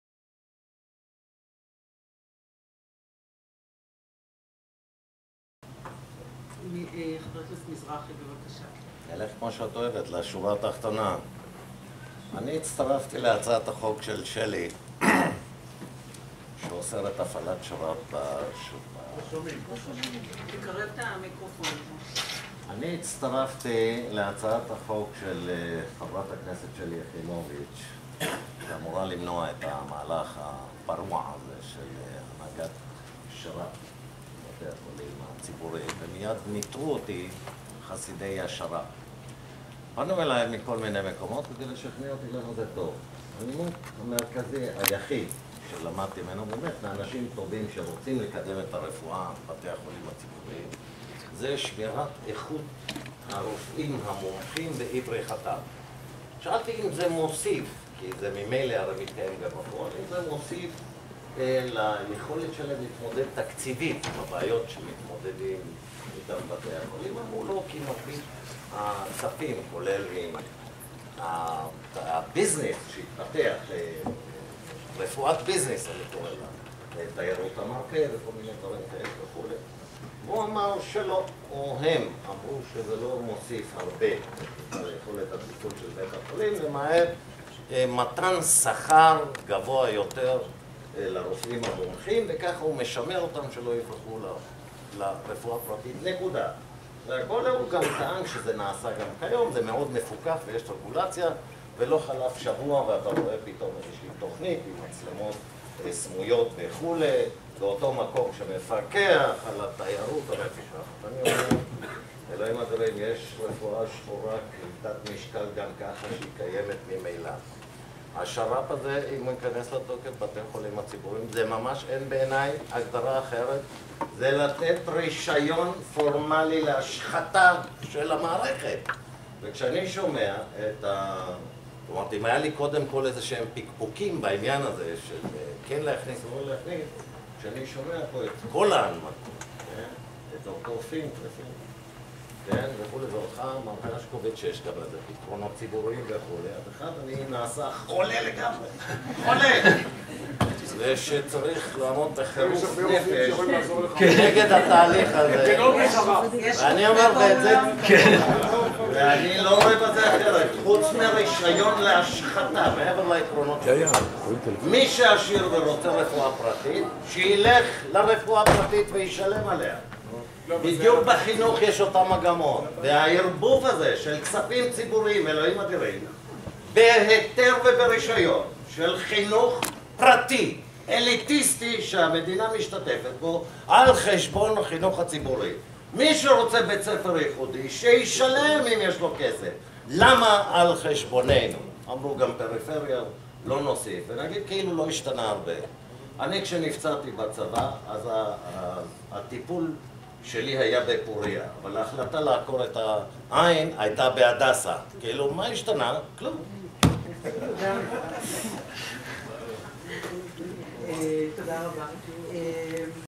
ח"כ משה מזרחי בכנס 'עוצרים את השר"פ'
ח"כ משה מזרחי (מפלגת העבודה), בכנס "שומרים על הבריאות - עוצרים את השר"פ" שארגנה ח"כ שלי יחימוביץ' בכנסת ב-04/03/2014.

"יש רפואה שחורה גם ככה, היא קיימת ממילא. השר"פ הזה, אם הוא ייכנס לתוקף בבתי החולים הציבוריים -- וממש אין בעיניי הגדרה אחרת -- זה לתת רשיון פורמאלי להשחתה של המערכת. אם היו לי קודם כל איזשהם פקפוקים בעניין הזה של לא להכניס או כן להכניס -- כשאני שומע פה את כל ההנמקות, אני נעשה חולה לגמרי. צריך לעמוד בחרוף נפש כנגד התהליך הזה."